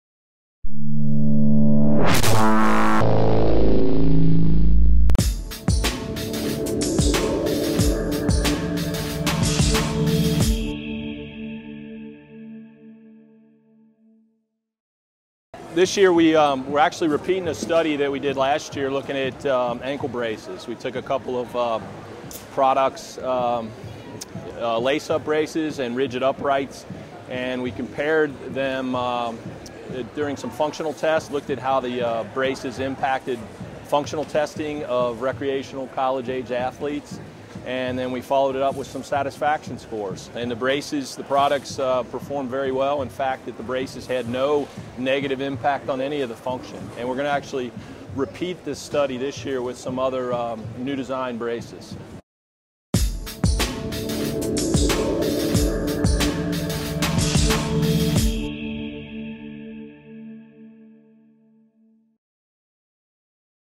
Does the new Active Ankle brace impact function?
Tony Brosky, PT, DHS, SCS of Bellarmine University, in Louisville, Kentucky has revitalized his ankle brace study from the previous TRAC meeting. Using both old and new designs of Active Ankle braces, he is testing lace up and ridged braces up against functional tests and measuring their impact amongst college-aged athletes.